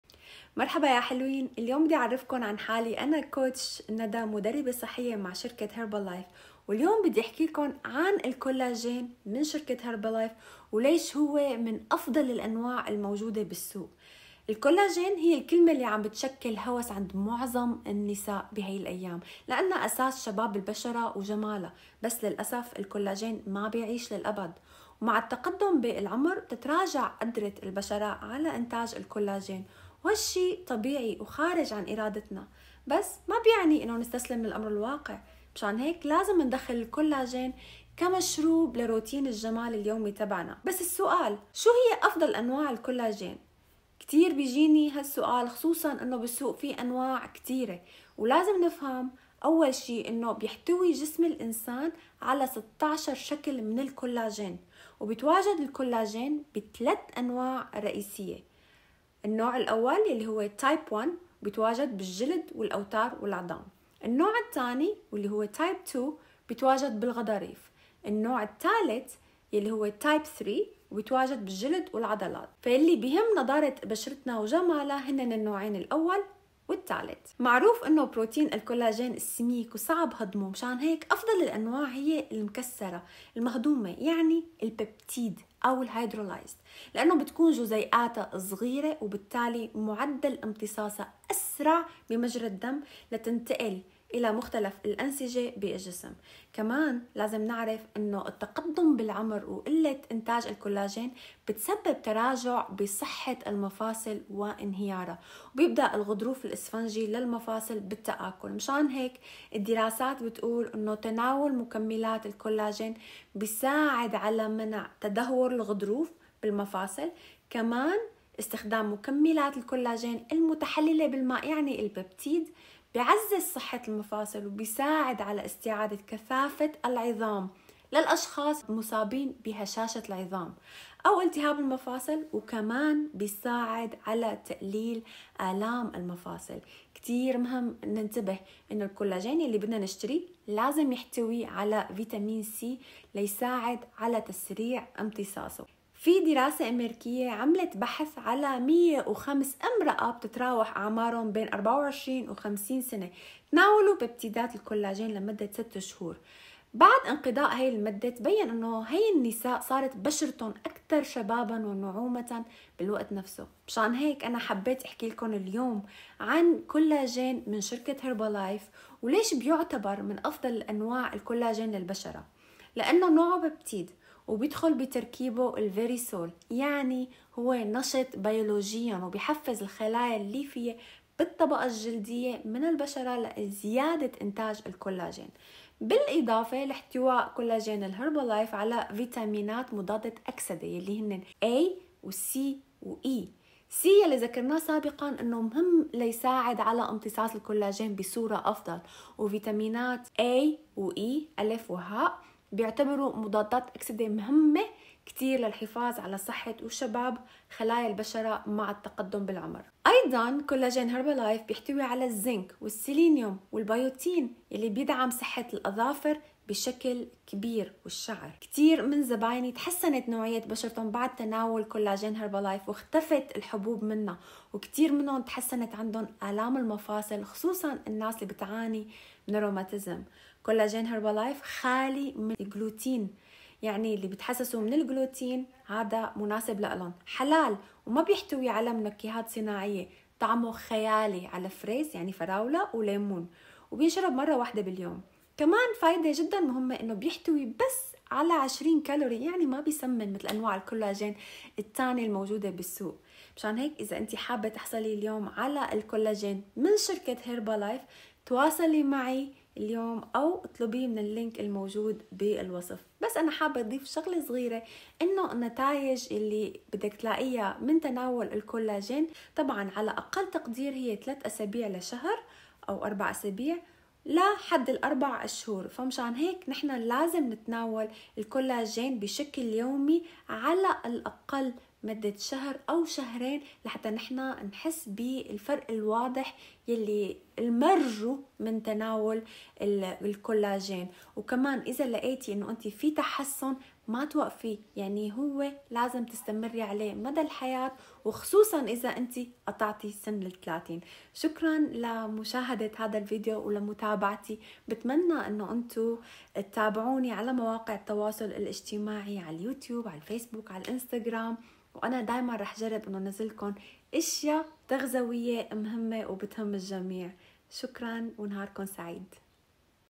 ليش كولاجين هيربالايف من افضل الانواع بالسوق
Herbalife SKIN® Collagen Beauty Booster: Strawberry Lemonade Canister 6.03 Oz.

الكولاجين معزز الجمال.. علبة عصير الفراولة والليمون 6.03 أونصة.

تمت صياغته مع كولاجين Verisol® *
لدعم مرونة الجلد وتقليل التجاعيد
الكولاجين هو ما يعطي الأدمة (الطبقة الداخلية السميكة لبشرتنا) هيكلها القوي. مع تقدمنا ​​في السن ، يتضاءل إنتاج الكولاجين في أجسامنا مما يؤدي إلى فقدان الثبات والمرونة وظهور التجاعيد.. يغذي الكولاجين البشرة من الداخل ويوفر العناصر الغذائية للحفاظ على بشرة شابة ومشرقة.
الفوائد الرئيسية
1- تم تركيبه باستخدام كولاجين Verisol® * الذي تم اختباره لإظهار دعم مرونة الجلد وتقليل التجاعيد الدقيقة.
2- ببتيدات الكولاجين النشط بيولوجيًا يمكن أن تقلل من علامات السيلوليت.
3- يحتوي على فيتامينات أ (مثل بيتا كاروتين) و C و E ، والتي تساعد على منع تلف خلايا الجذور الحرة الضارة التي تؤدي إلى شيخوخة بشرتك.
4- يدعم الأظافر القوية والشعر الصحي بالسيلينيوم والزنك والبيوتين.